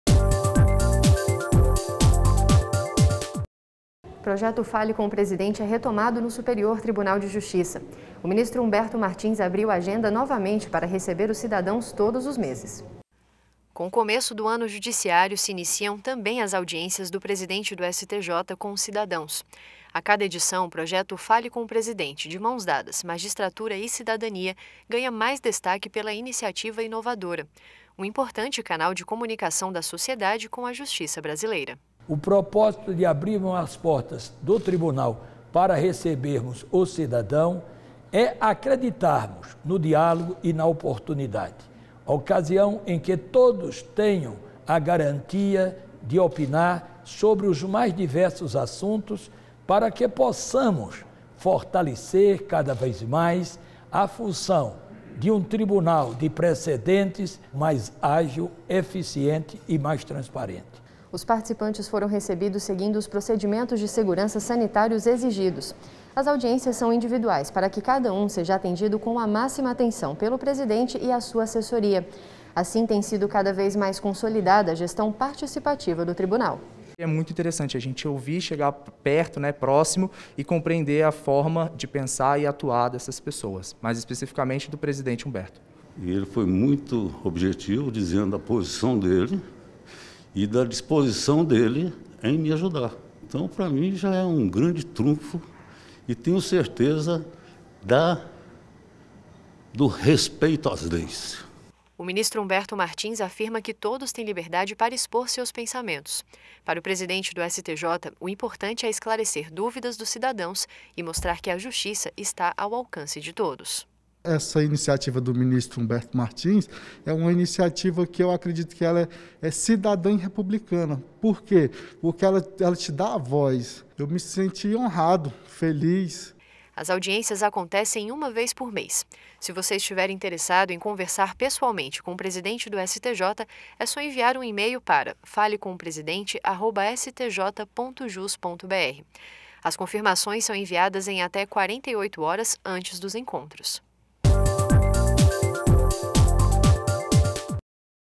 Ministro Humberto Martins recebe público dia 22 de fevereiro no Projeto 'Fale com o Presidente'
​​​​Dando continuidade à missão de aproximar o Judiciário da população brasileira, o presidente do Superior Tribunal de Justiça (STJ) e do Conselho da Justiça Federal (CJF), ministro Humberto Martins, receberá, neste mês, cidadãos de todo o país na 4ª edição do projeto Fale com o Presidente – De mãos dadas: magistratura e cidadania.